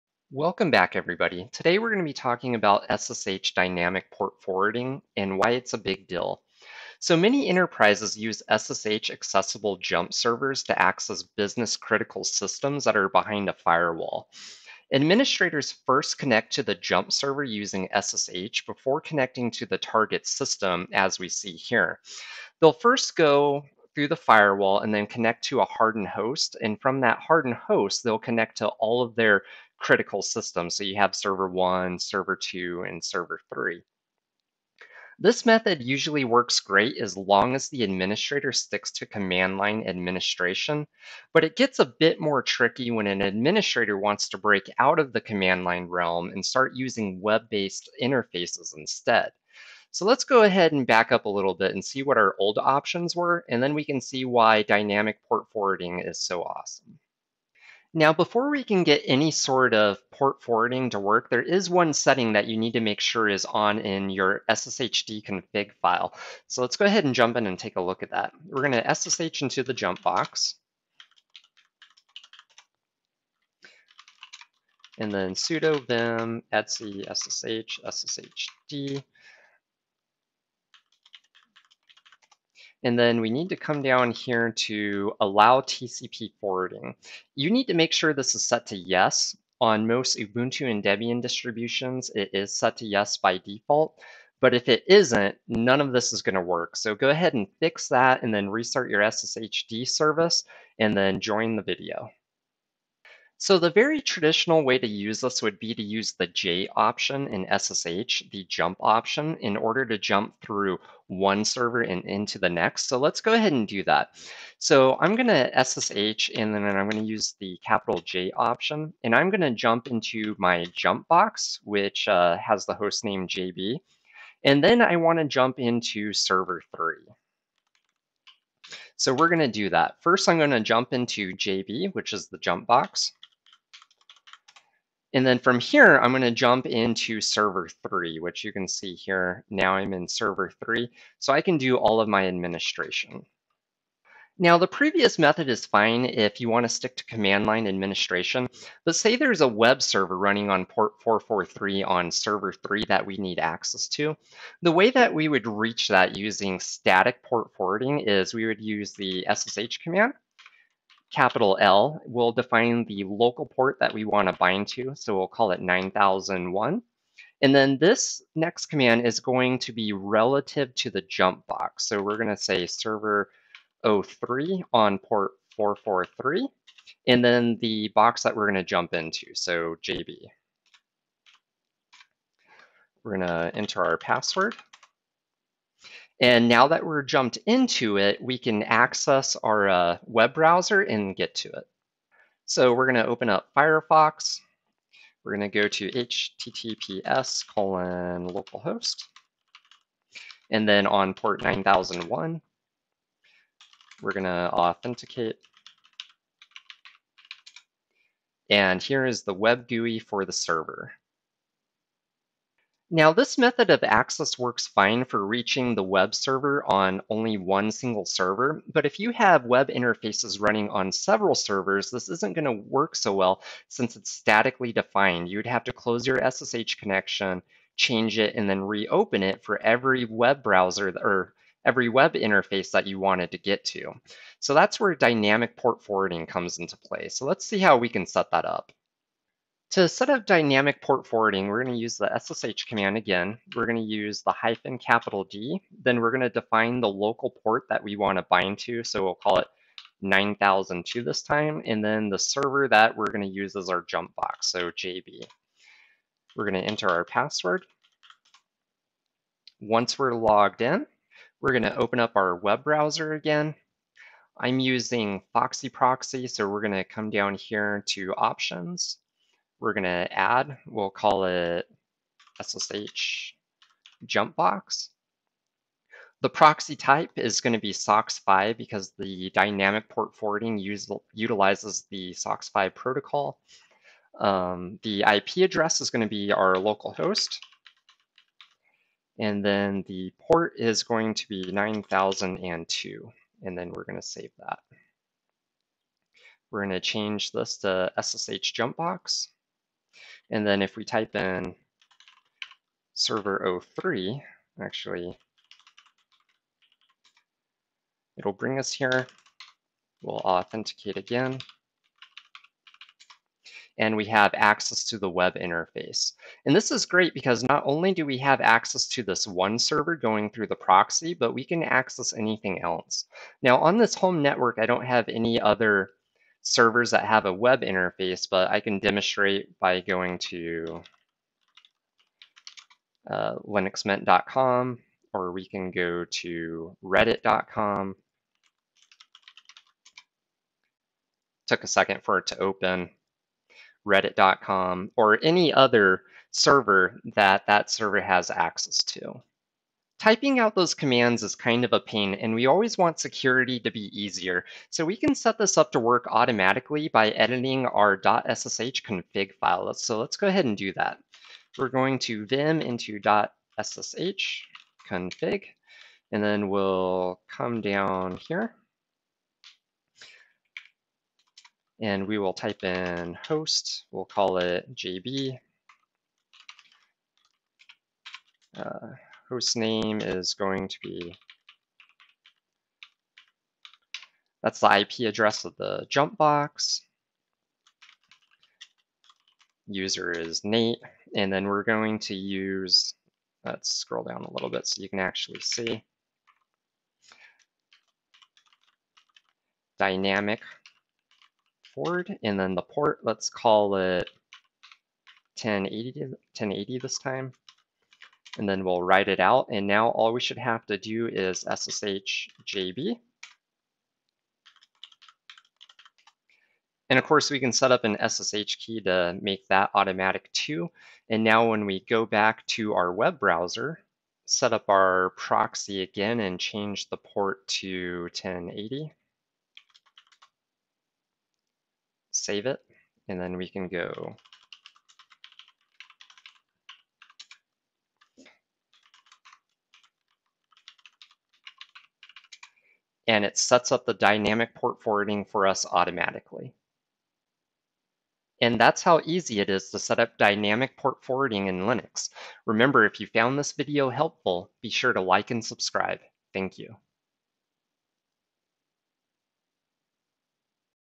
SSH Dynamic Port Forwarding
In this video we go over how to use SSH dynamic port forwarding in Linux.

BTC - 3NzWDDH3n5PsBKqWJyAb7rvsGvJKxcAPFv
ETH - 0xB09212235c648a448F0E7eEB416ce5652362c772

Local port forwarding example:
$ ssh -L 9001:localhost:443 jumpbox

Dynamic port forwarding example:
$ ssh -D 9002 jumpbox